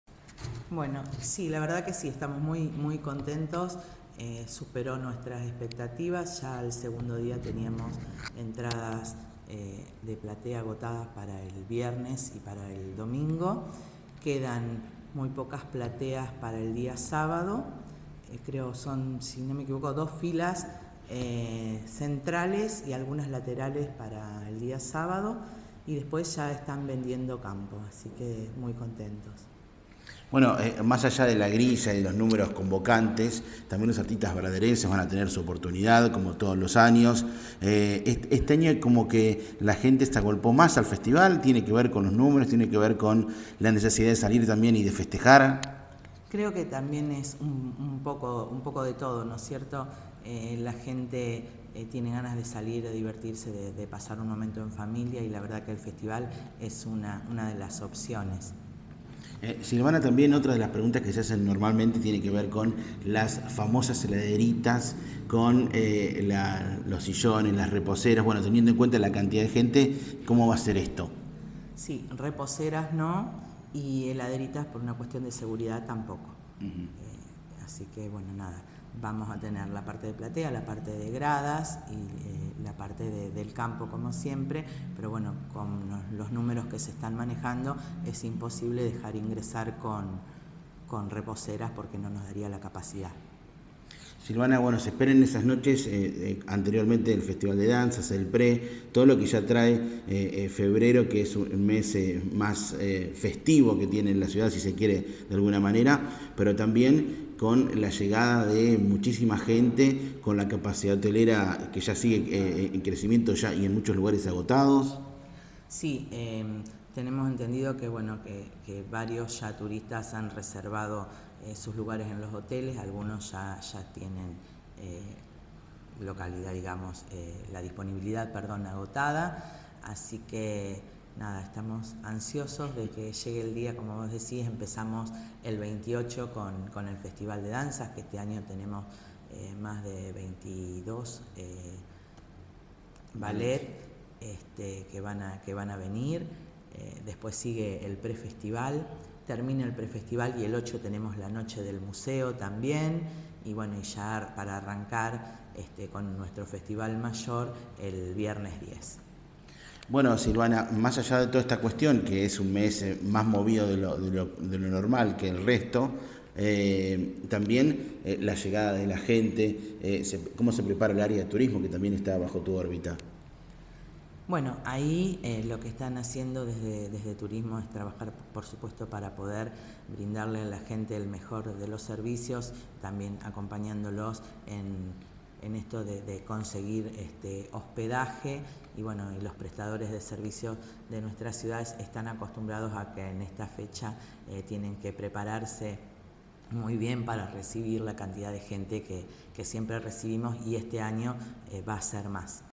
FESTIVAL , VENTA DE ENTRADAS Y TURISMO- SILVANA IOZZIA
LA SECRETARIA DE CULTURA EDUCACION TURISMO Y DEPORTE HABLO DEL FESTIVAL LA VENTA DE ENTRADAS Y EL TURISMO

Silvana Iozzia dialogó con Marcelo Zich para Th producciones sobre el furor por el festival, las entradas agotadas para el viernes y el domingo y además analizó lo que pasa con la llegada de turismo en la época mas festiva de la ciudad.